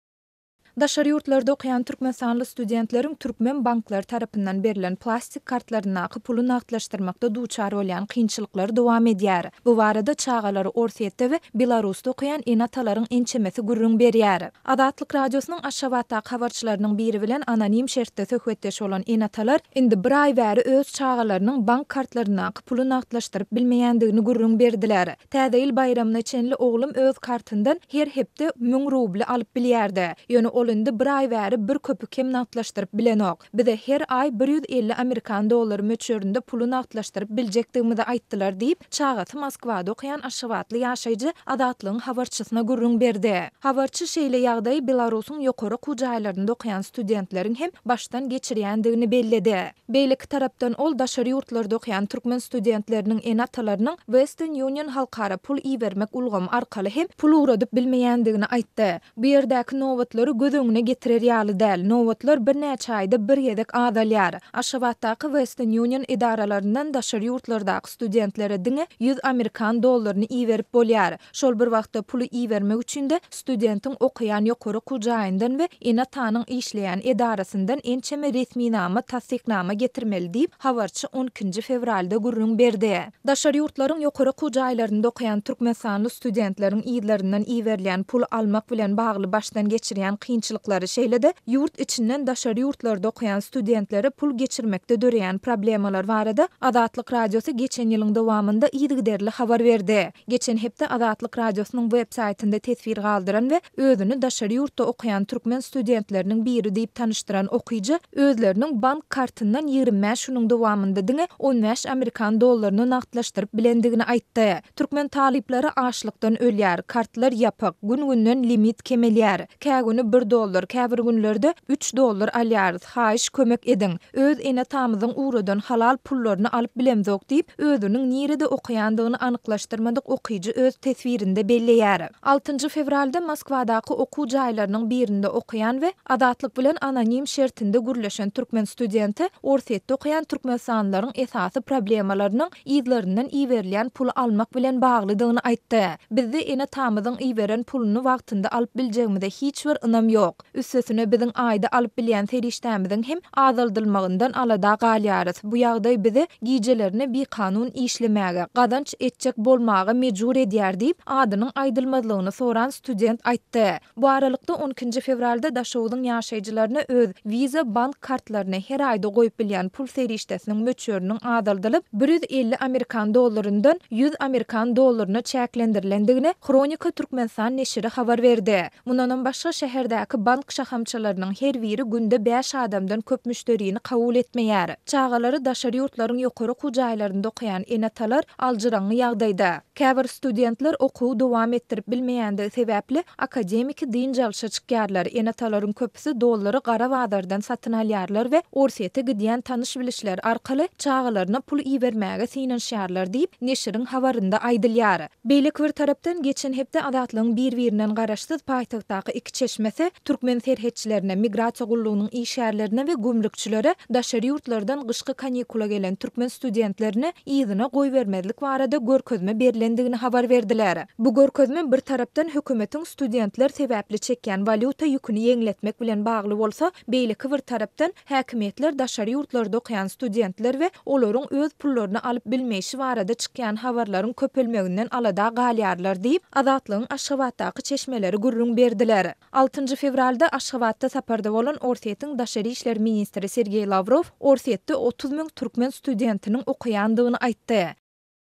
Russiýada we Belarusda okaýan türkmenler bir aý bäri pullaryny nagtlaşdyryp bilmeýärler
Daşary ýurtlarda okaýan türkmenistanly studentleriň türkmen banklary tarapyndan berlen plastik kartlaryndaky puly nagtlaşdyrmakda duçar bolýan kynçylyklary dowam edýär. Bu barada çagalary Orsýetde we Belarusda okaýan ene-atalaryň ençemesi gürrüň berýär.